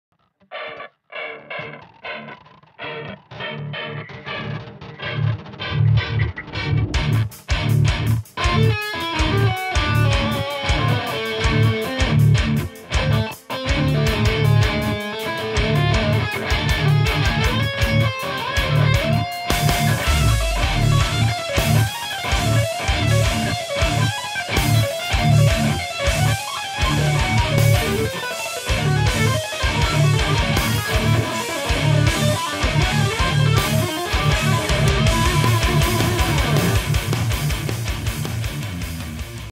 Guest solo for Ron George
Tried real hard not to rip off Allan Holdsworth and Fredrik Thordendal, but it is what I felt and what the rhythms called for... Hope you enjoy.
Gear
.Strandberg* Boden guitars
EMG 707-x and 81-7x pickups
Toontrack software
Presonus interface
Cubase DAW